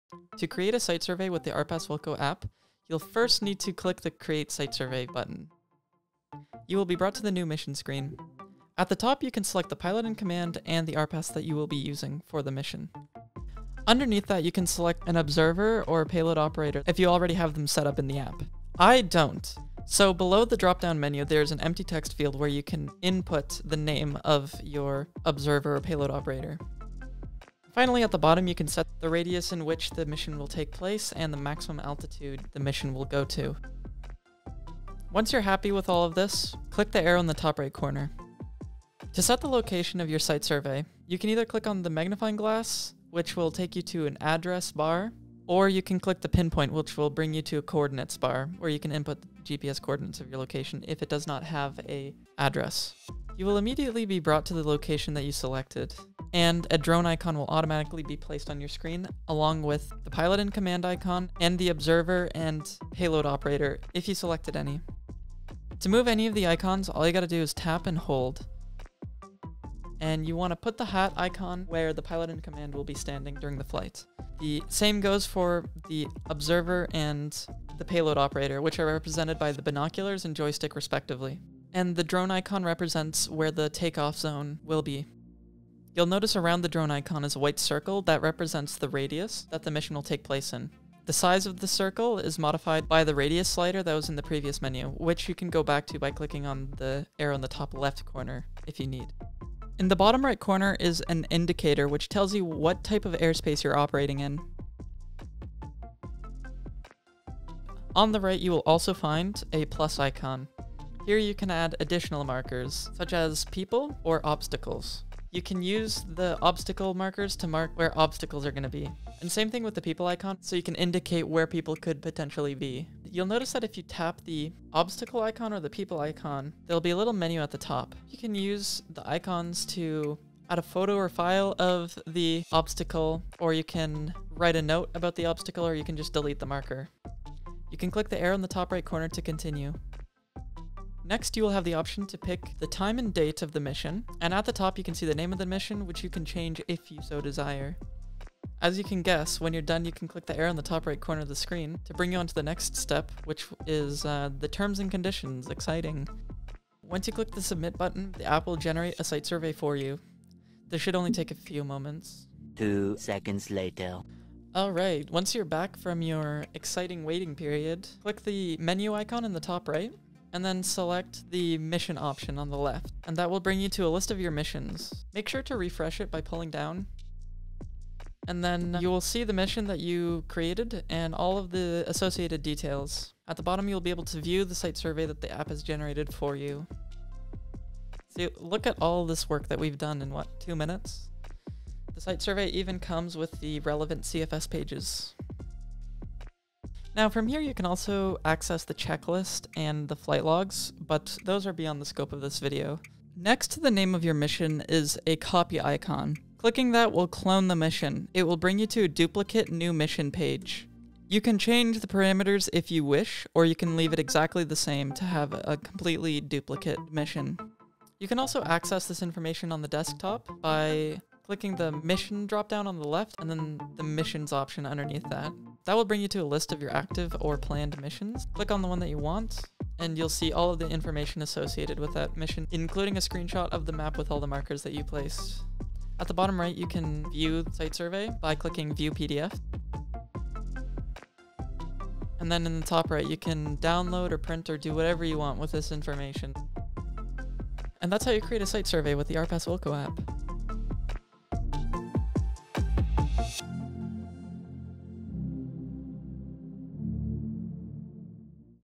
Make a site survey in 2 minutes using RPAS WILCO
Making a site survey is annoying, especially considering how often you have to do it. I hope this can help make the process less inconvenient for you all :)

RPAS WILCO is the only authorized Drone-as-a-Service provider among NAV CANADA aeronautical data distributors.

0:00 - New mission
0:53 - Crew and RPAS markers
1:42 - Airspace indicator
1:52 - Additional markers
2:20 - Date and name
2:29 - Submission
2:44 - Viewing the site survey